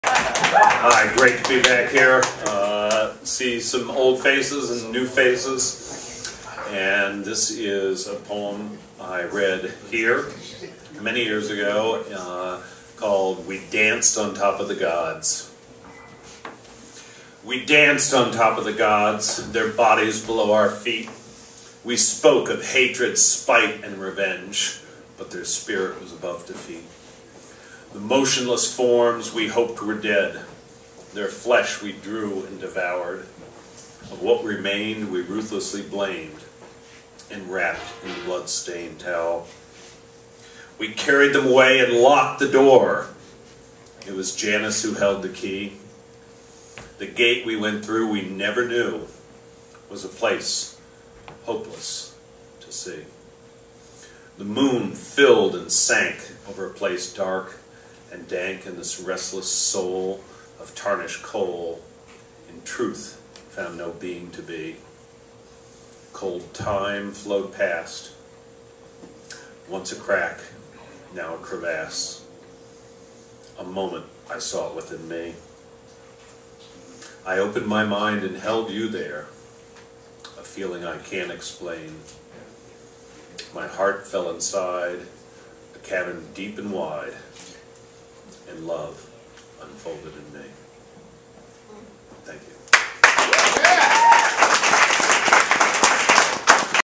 Scott Knox - We Dance On Top Of The Gods
Scott Knox performing "We Dance On Top Of The Gods" at Nitty Gritty Slam #61 at Valentine's on Tuesday, January 7, 2014.

More info on upstate New York poetry and spoken word